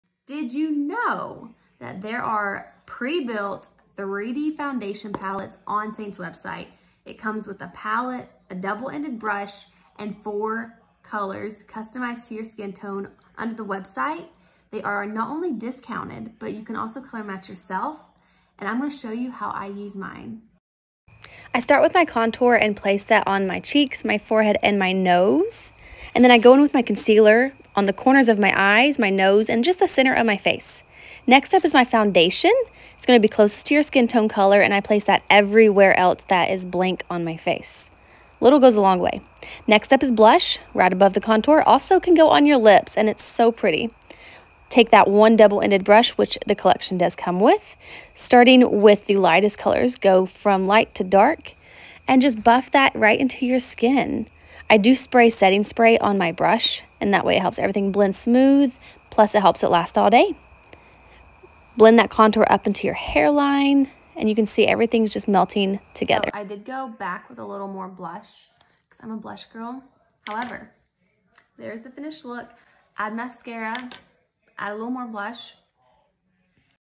Prebuilt full face makeup palettes?!
Did you know that this was an option?!
It is. You can TOTALLY skip sending me a makeup free selfie if the thought of that freaks you out!
Comment COLLECTIONS and I’ll send you a direct link to pick yours!
When you arrive at the collections page, click on “find your shades” which is a shade selection guide.
For reference, I wear the level IV collection. And don’t worry about not knowing what to do once you get it. I am going to send you an email with a real-time tutorial!